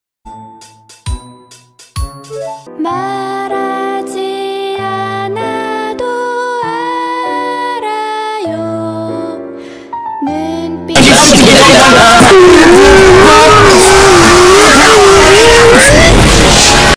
2022
2022년도에 저한테 일어난 일들을 16초로 요약해서 만든영상임을 알립니다.